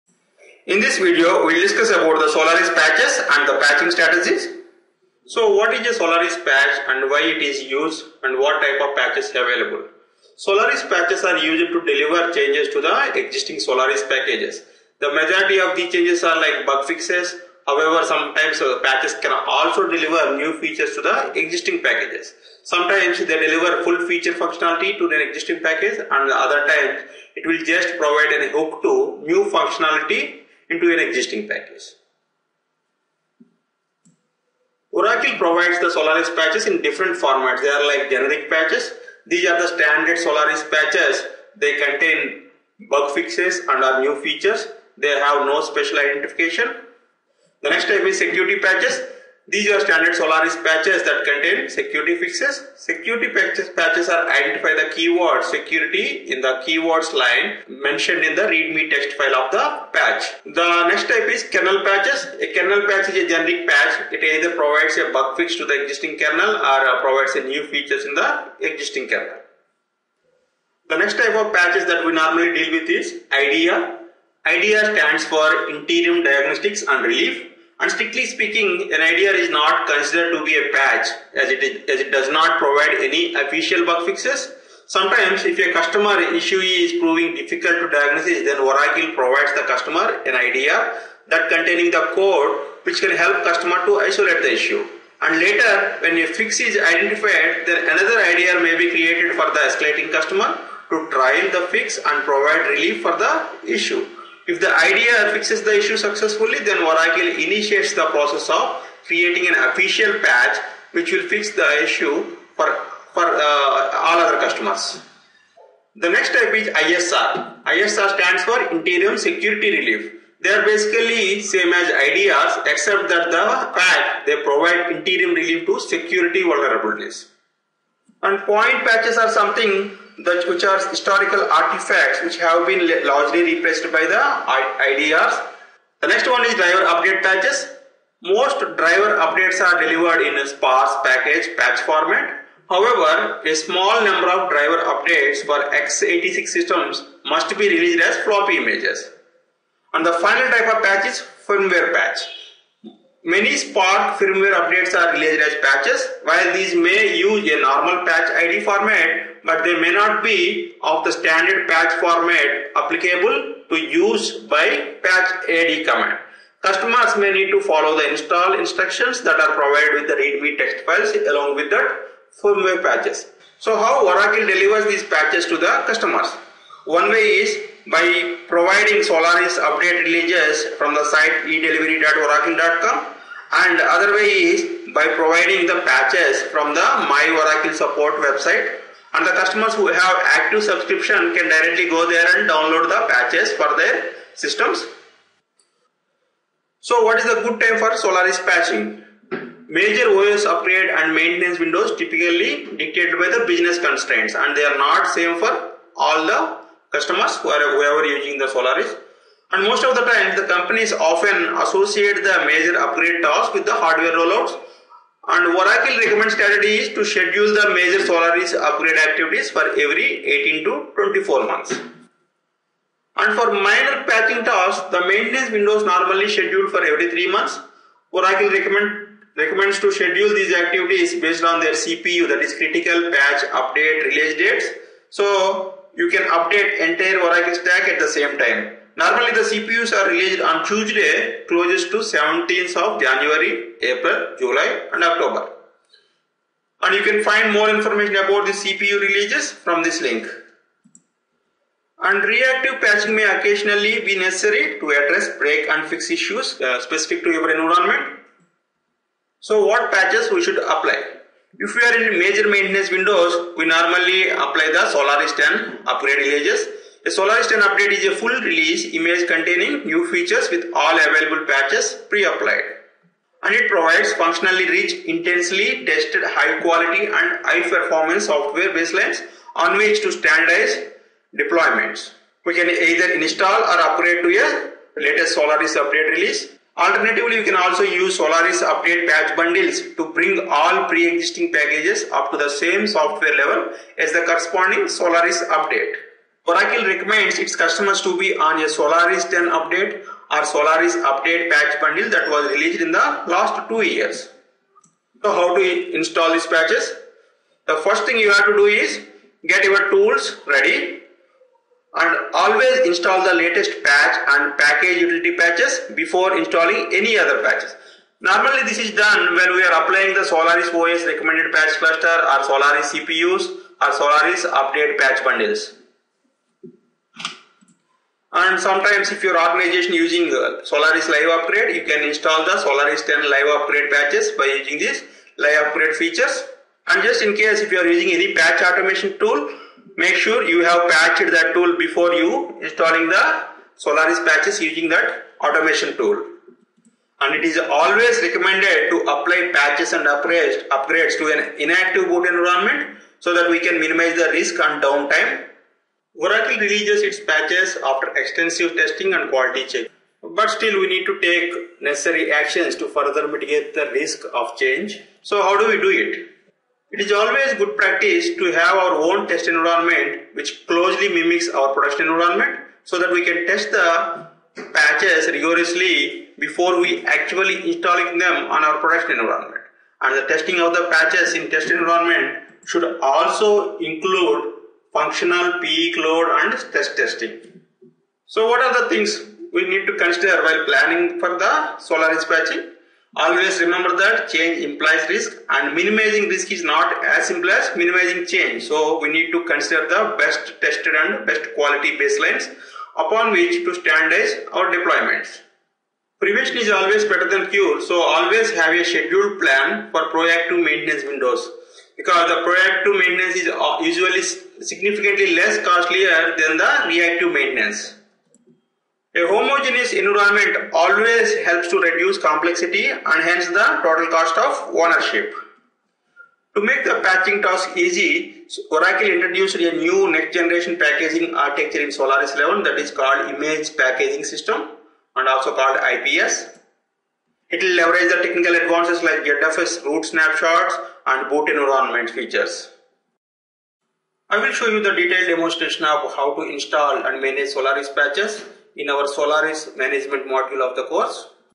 Solaris Patching Strategy
System patching one of the important job responsibility for Solaris Administrator. And when we are doing it online,  it involves much more than just running patchadd commands. In this video we are taking you through the Recommended Solaris Patching Strategy for the Solaris Environment